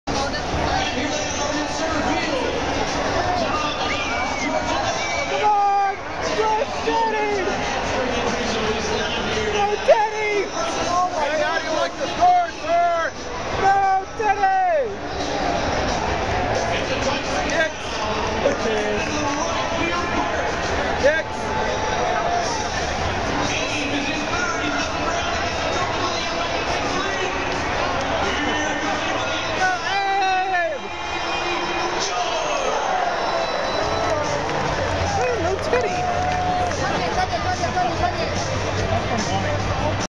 President's Race - Washington Nationals 09/22/09 Natstown
Natstown President's Race at the Washington Nationals ballpark on 09/22/09.  Once again no Teddy!  Boooooooo!

Kodak Zi8.  720p at 30fps.